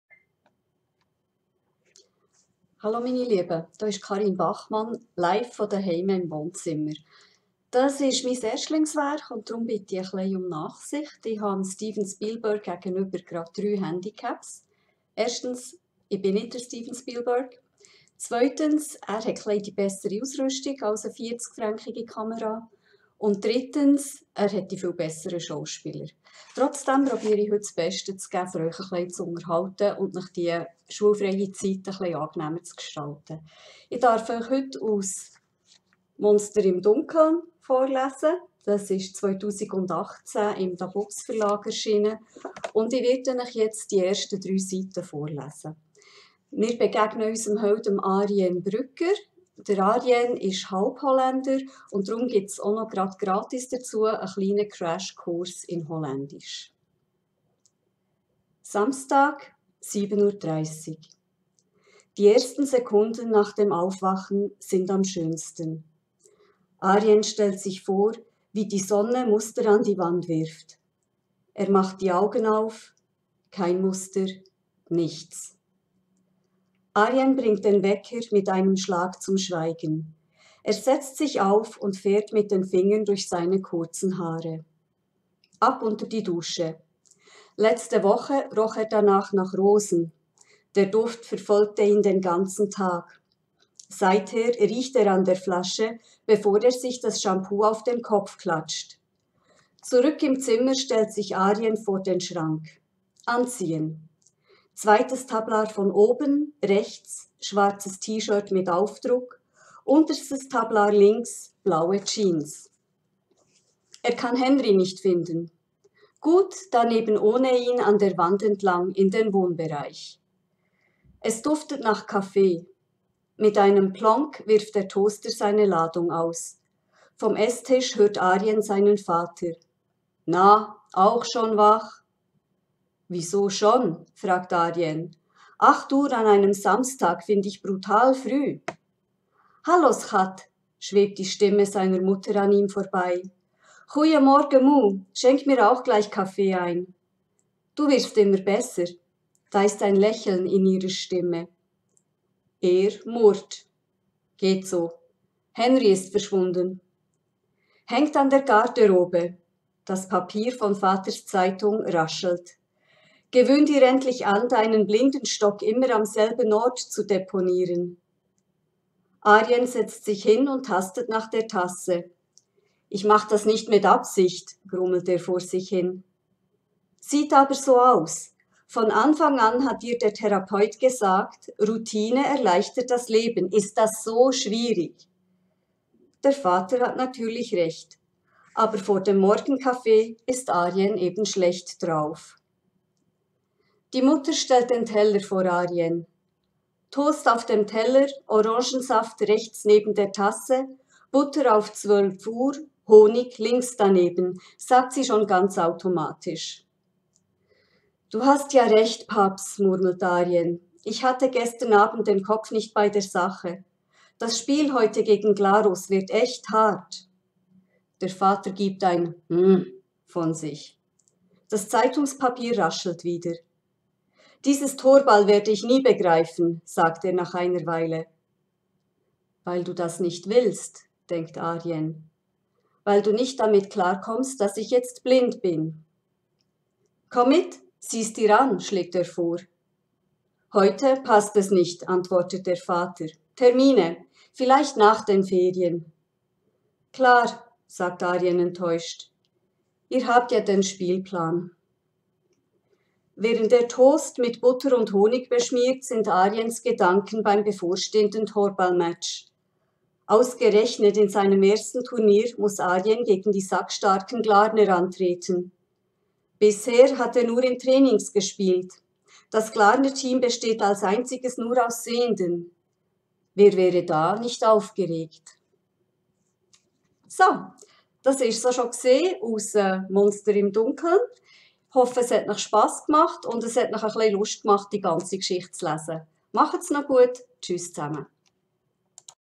ab 12 Jahren – Karin Bachmann liest aus ihrem Buch "Monster im Dunkeln"
"Ein brutaler Psycho ist hinter Arjen her. Seine Eltern sind nicht da. Zur Polizei traut er sich nicht. Im Dunkeln lauert die Angst. Denn Arjen ist blind."

"Monster im Dunkeln" ist 2018 im dabux Verlag erschienen. Karin Bachmann liest in diesem Video die ersten drei Seiten